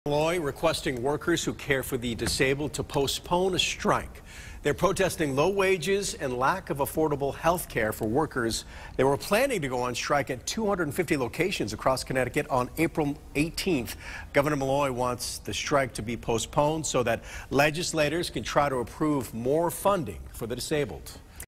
Malloy requests workers who care for disabled to postpone strike
Workers are protesting low wages and a lack of affordable healthcare for workers. They were planning to go on strike at 250 locations across Connecticut on April 18.